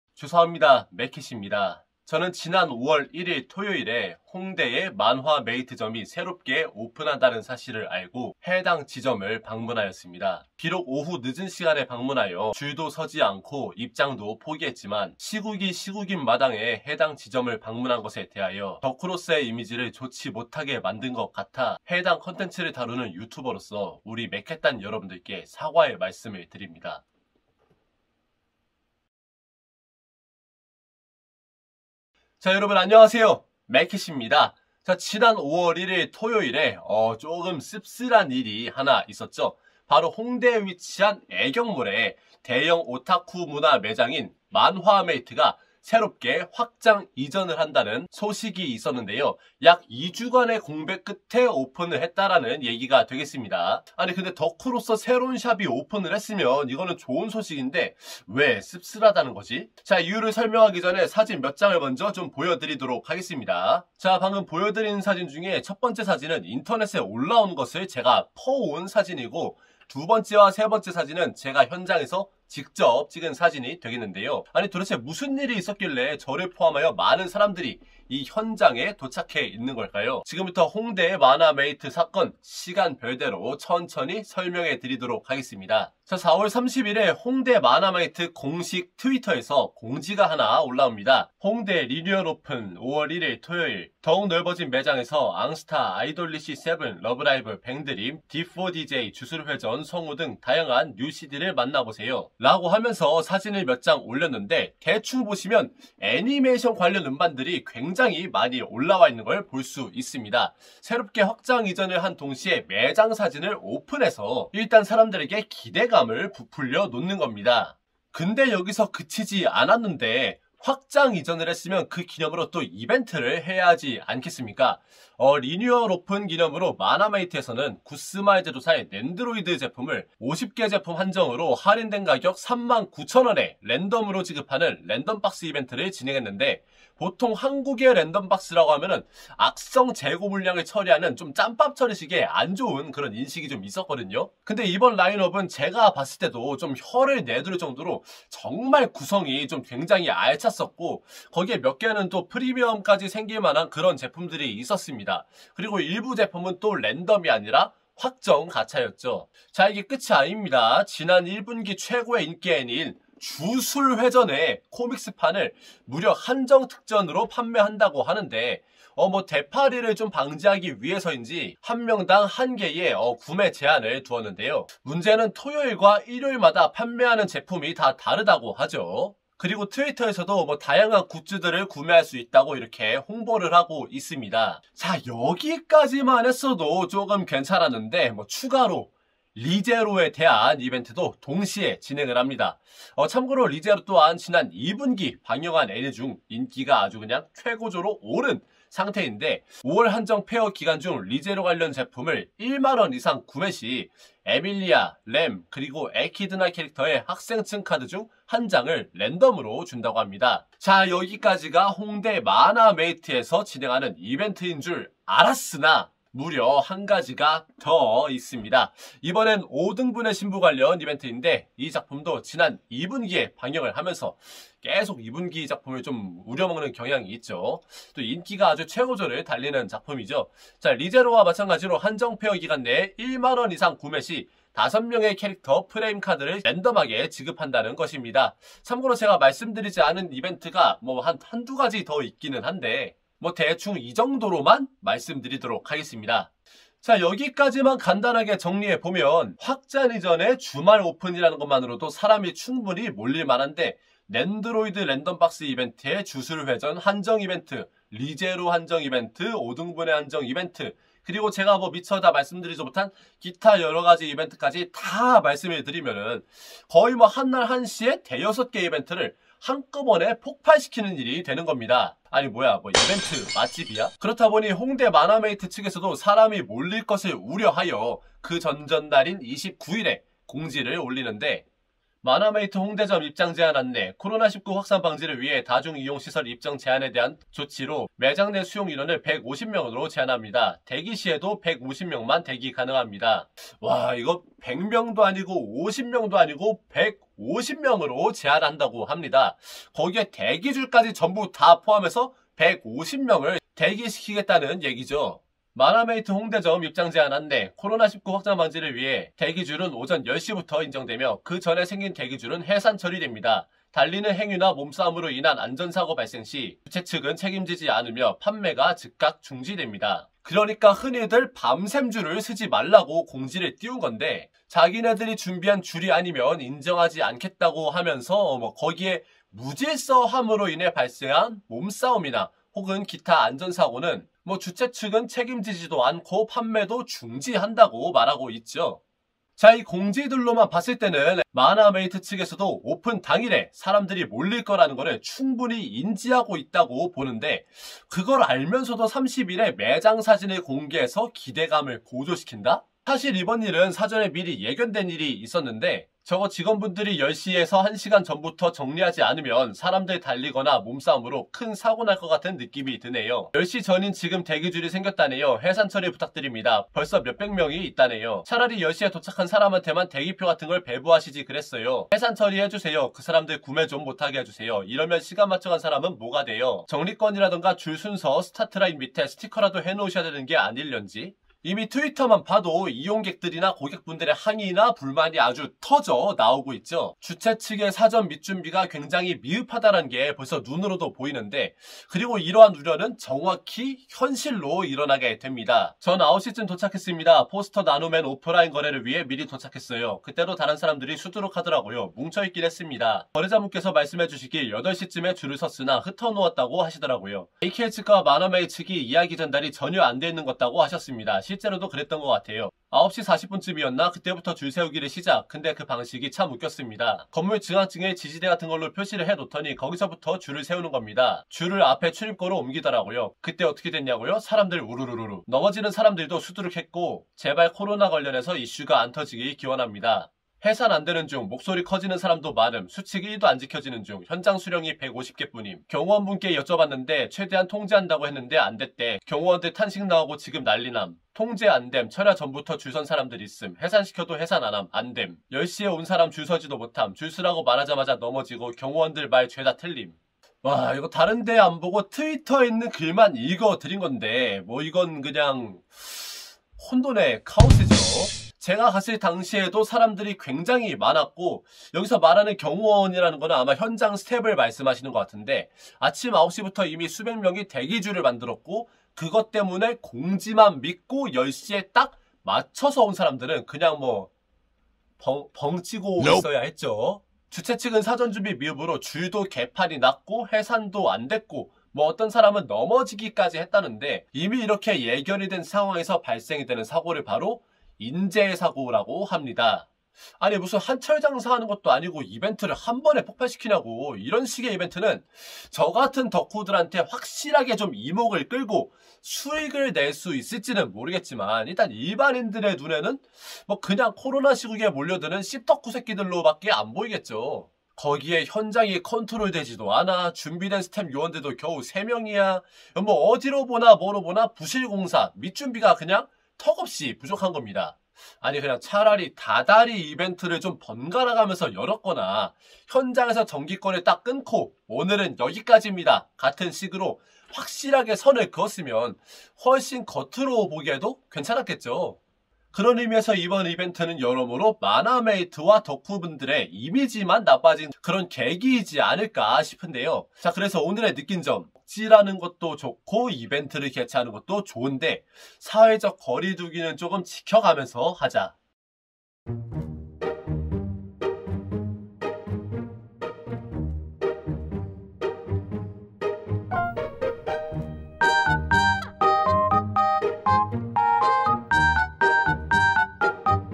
씹덕 유튜버 소신발언!! 홍대 애니X이트 실망이다. 코로나 시국에 몰려든 수백명의 덕후들!! 논란과 사건사고 총정리!!
오늘은 지난 05월 01일 (토요일) 에 있었던 홍대 덕질장소인 애니X이트 에
수백명의 인파가 몰렸던 사건사고 를 총정리 해 봤습니다.